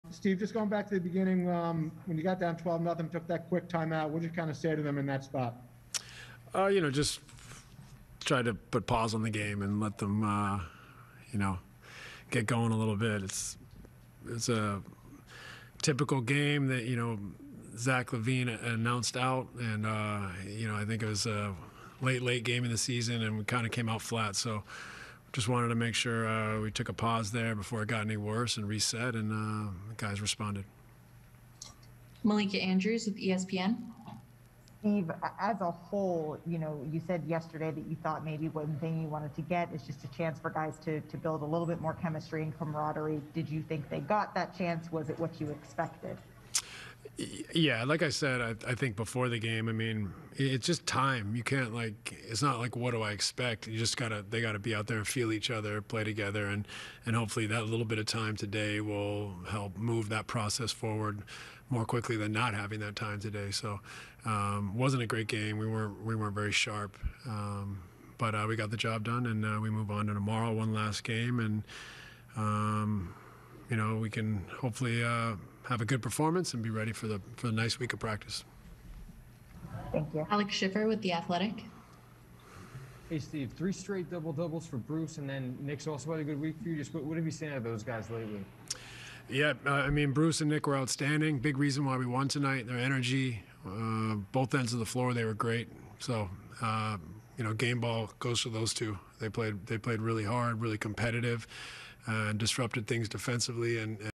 Steve Nash met with the media to discuss the Nets' blowout win over the Bulls.
Nash talks bench energy, the slow start, and the 105-91 win over Chicago.